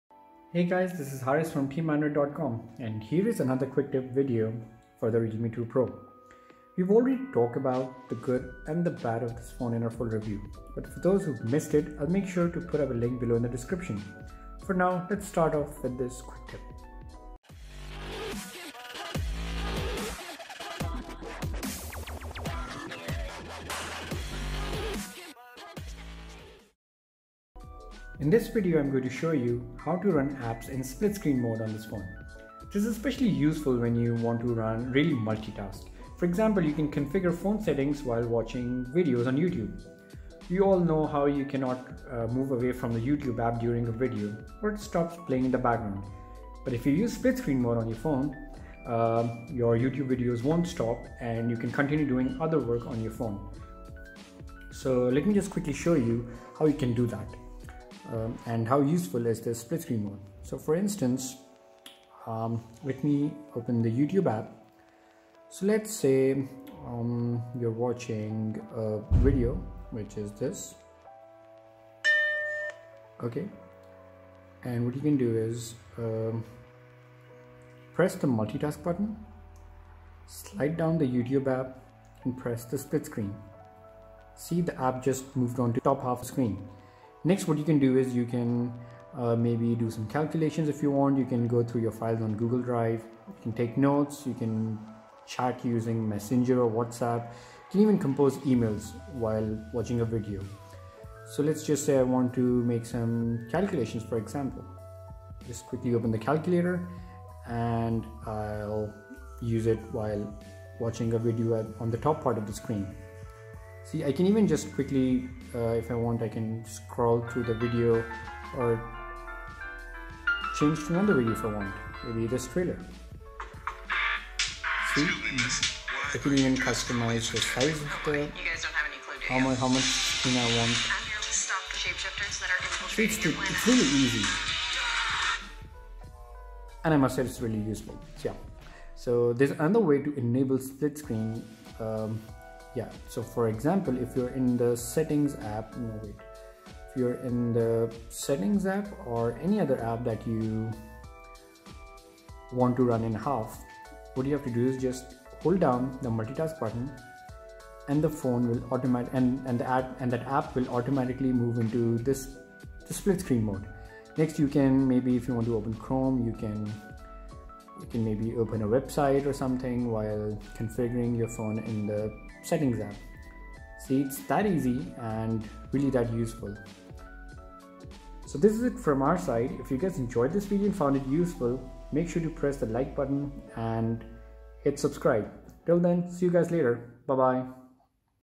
How to Run Apps in Split-Screen on Realme 2 Pro
In this video, I’m going to show you how to run apps in split-screen mode on this phone.

This is specially useful when you want to really muti-task. For example, you can configure phone settings while watching videos on YouTube. We all know you can’t move away from the YouTube app during a video or it stops playing in the background, but if you use split-screen, your video will not stop and you can perform other actions on the Realme 2 Pro.